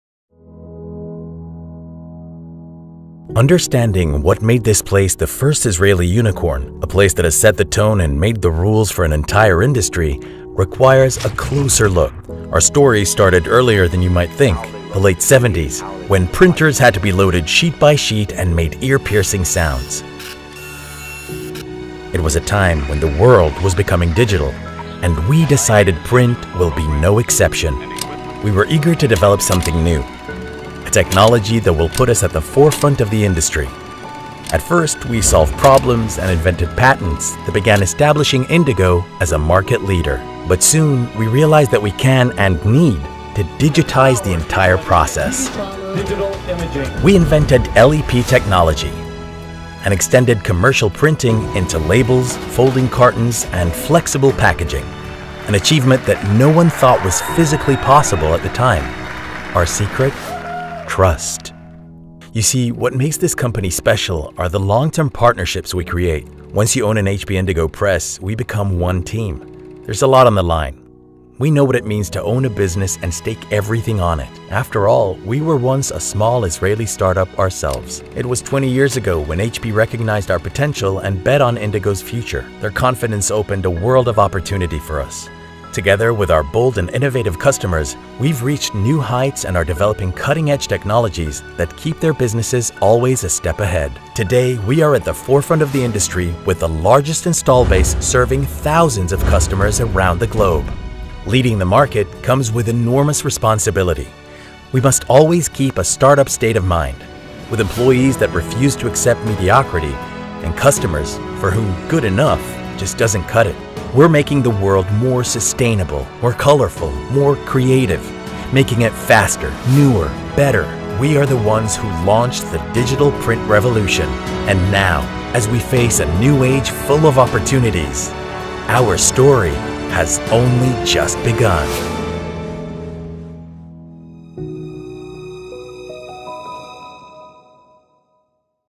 History 101: How HP Indigo launched the digital revolution
Since its inception as a small Israeli startup, the HP Indigo brand has set the tone and made the rules for the entire digital print industry.

This short, but fascinating video takes viewers back to the late 70s, “when printers had to be loaded sheet-by-sheet and made ear-piercing sounds.”

Today, HP Indigo remains at the forefront of the digital printing revolution, serving 1000s of customers in over 180 countries worldwide.

Watch the video to see what more is in store!